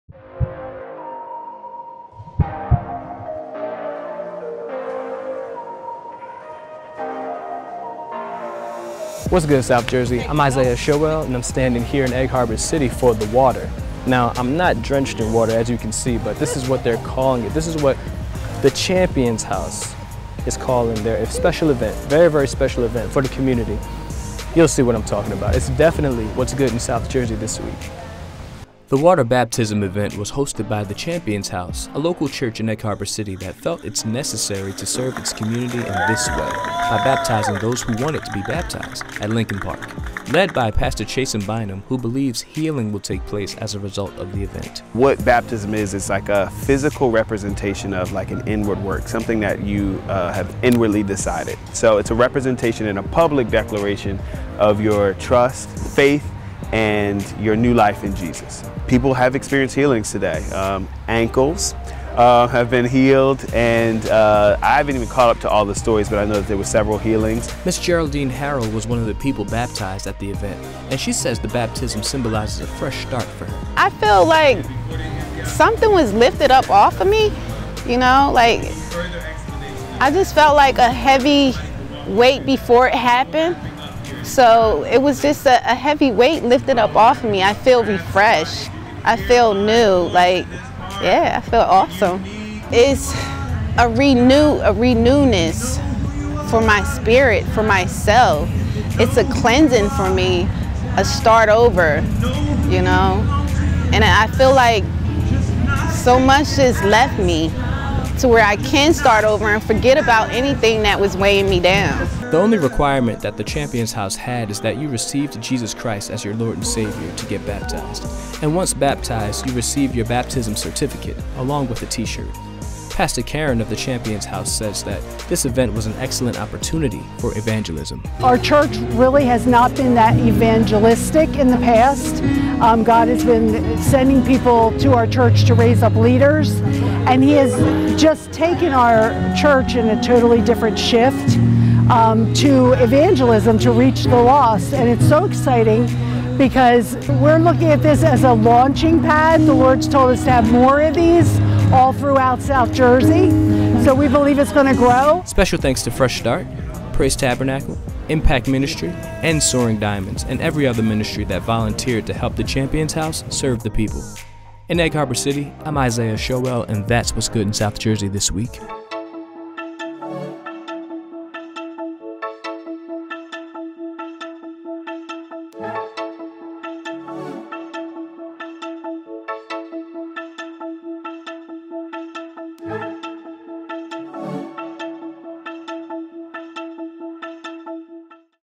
Water Baptism Champions House
For Egg Harbor City The Champions House along with other churches baptized some people in the community looking to serve and evangelize with praise & worship available along with participants receiving a certificate marking the day they got baptized, it was definitely what's good in south jersey this week!